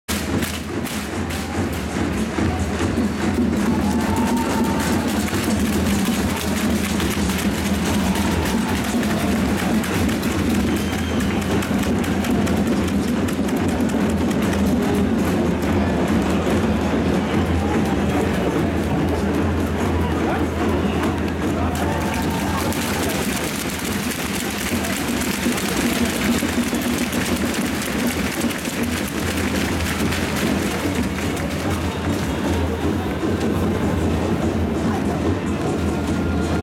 20210321 第6節 サガン鳥栖vsアビスパ福岡 ウォーミングアップ開始時
５年ぶりのの九州ダービー。盛り上がりの様子。